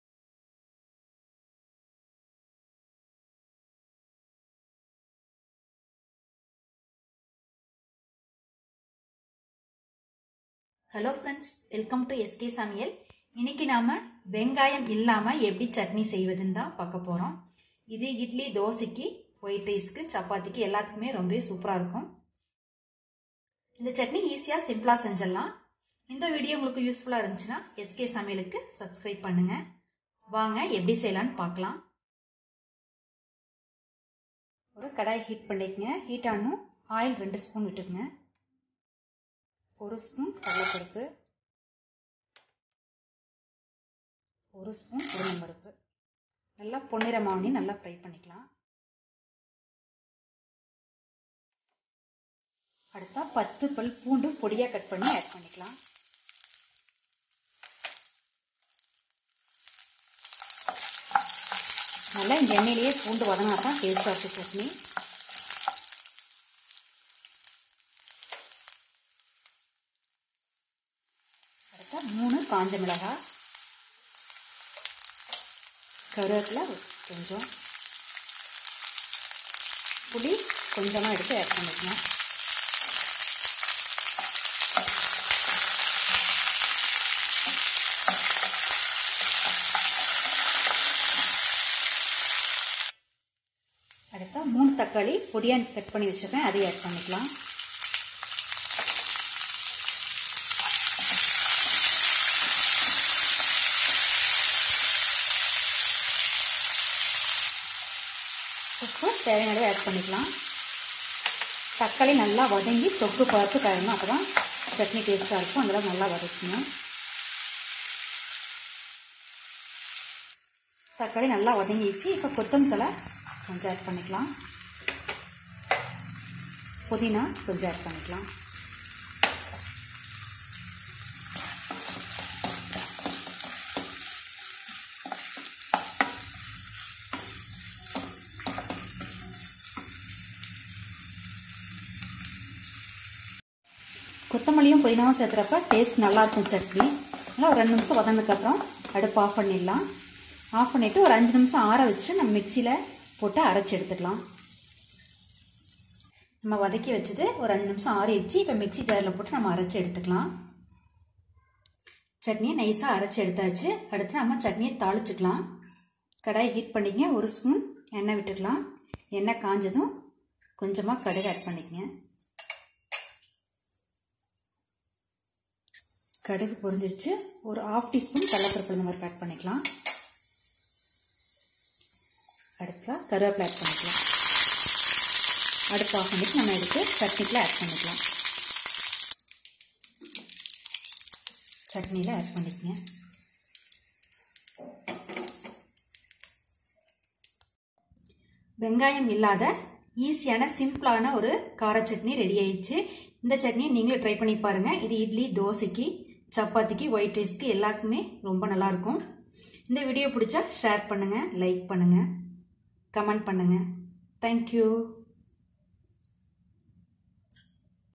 No onion chutney/kara chutney/வெங்காயம் இல்லாத சட்னி|No onion recipe/tomato chutney
வெங்காயம் இல்லாமல் சட்னி செய்வது எப்படி/No onion chutney/kara chutney/ No onion recipe/tomato chutney/காரச் சட்னி

Ingredients

1. Urad dal    - 1 tbs
2.channa dal   - 1 tbs
3.garlic             - 10 no
4.red chilly       - 3
5.curry leaves  - as needed
6. tomato           - 3
7. tamarind
8. coriander leaves
9. pudina leaves
10.salt   as required